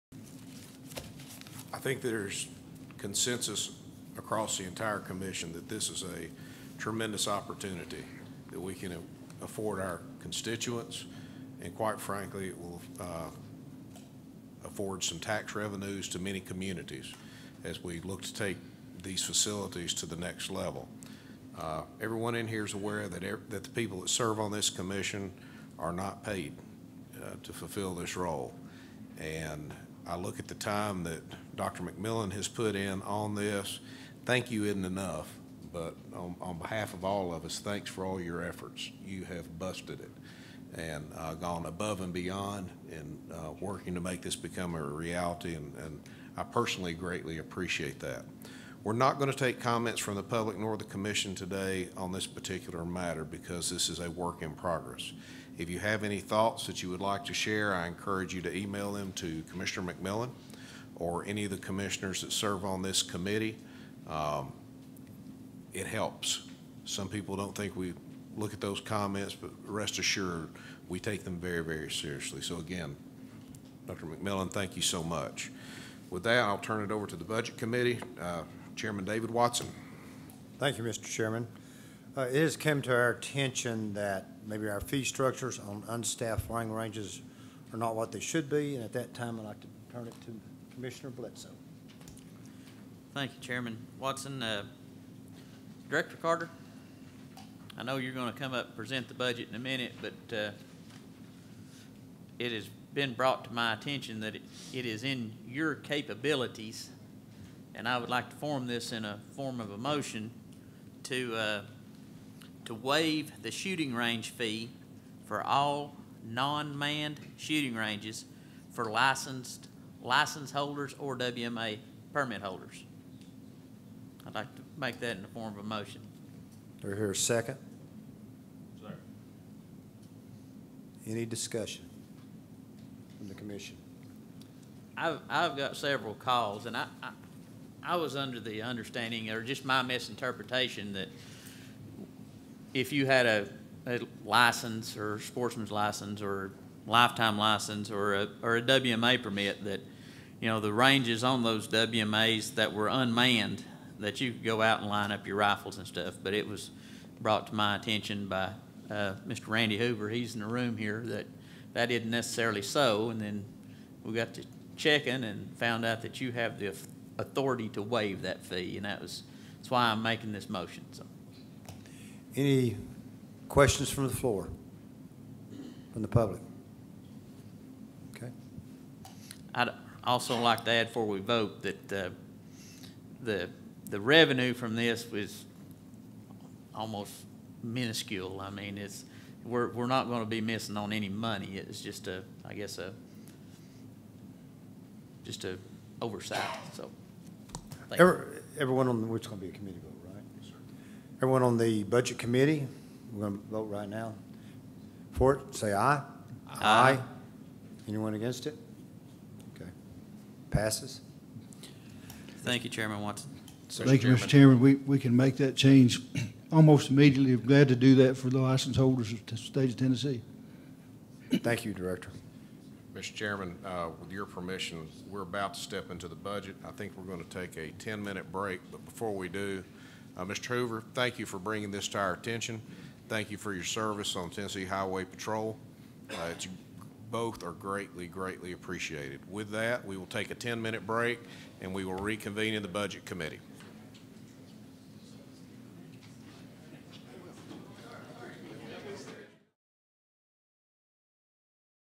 TFWC Committee Meeting 081816.2
Part 2: Agency Budget Presentation

August 18, 2016 Committee Meeting of the Tennessee Fish & Wildlife Commission held in Crossville, TN.